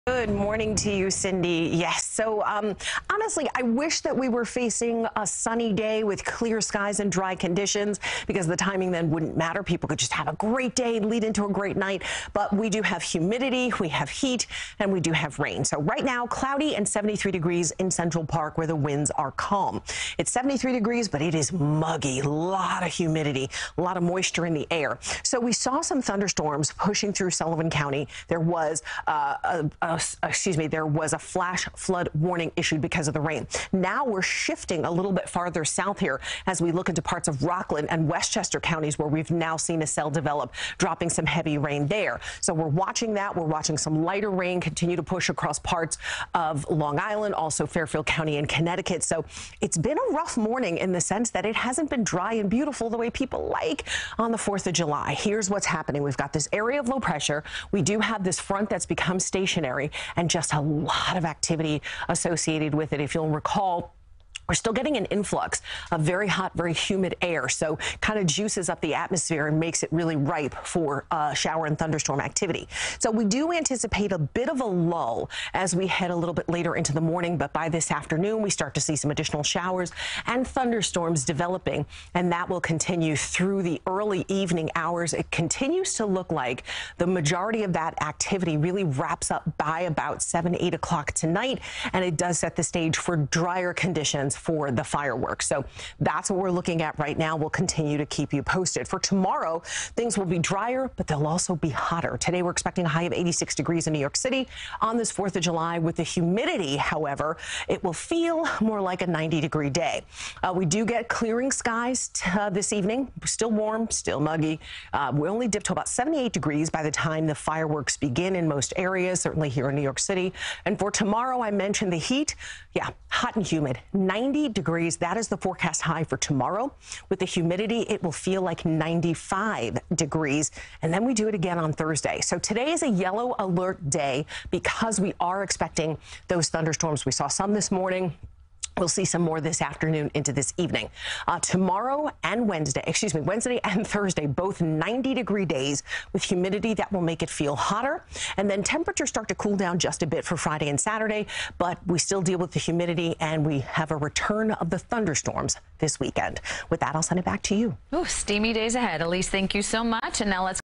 First Alert Weather: Yellow Alert on July 4th
Pop up thunderstorms and showers may interfere with your holiday plans. CBS New York's Elise Finch has a look at the forecast.